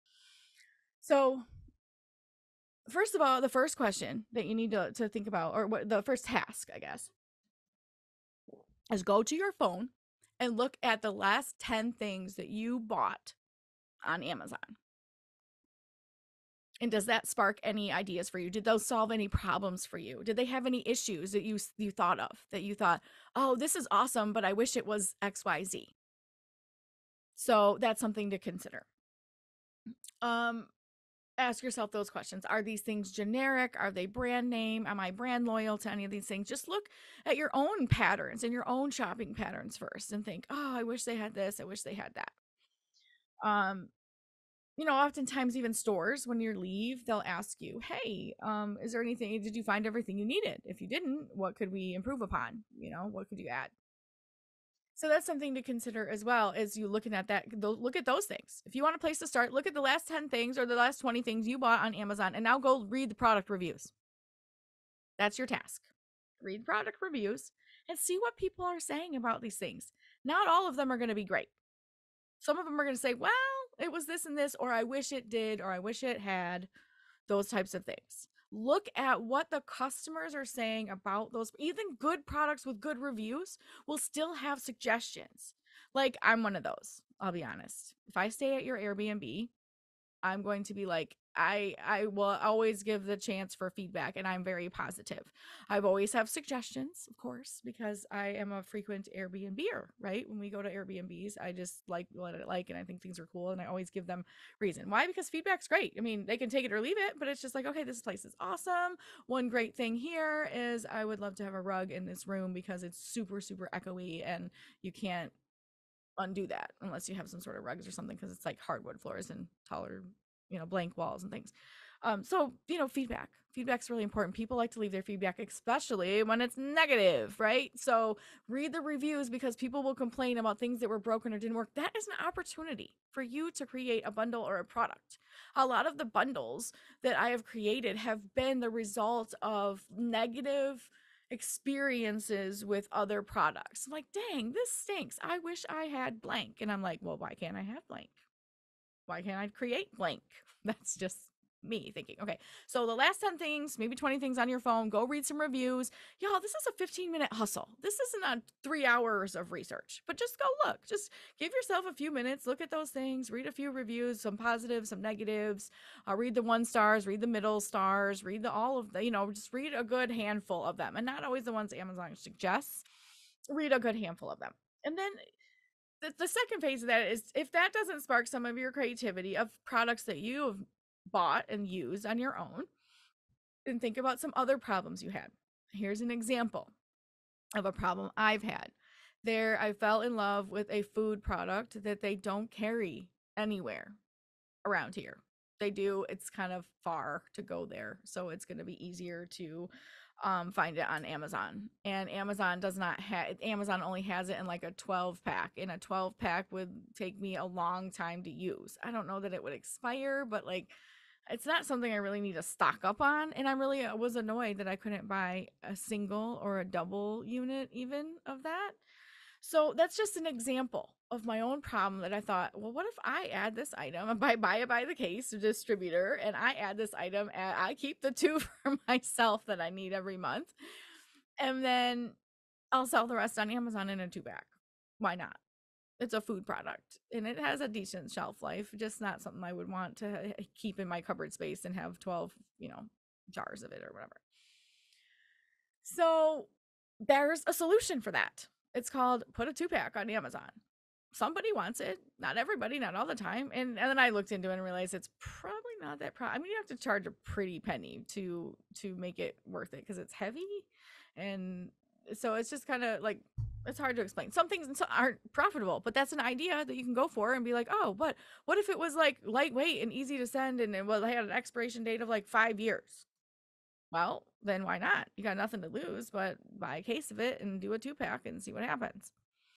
From Customer to Seller: Transforming Your Amazon Shopping Experience into Business Opportunities
Discover how to unlock Amazon product ideas using your own shopping patterns! This video will guide you through using your past Amazon purchases, reading product reviews, and leveraging feedback to find unique product and bundle ideas. Tune in for practical tips and real-life examples.